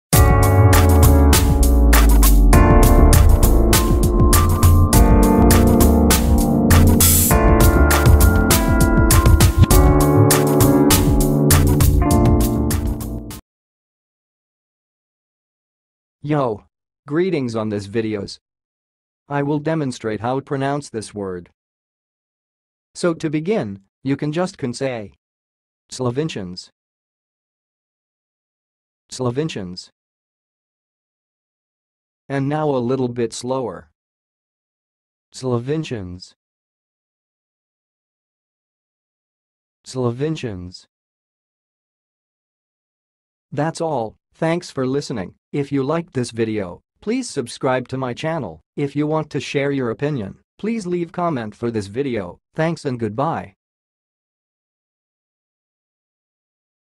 SLOVINCIANS - HOW TO PRONOUNCE IT!?
Watch how to say and pronounce "slovincians"!
Want to know how other words sound like? Look for slovincian, slovincians on wordbox channel!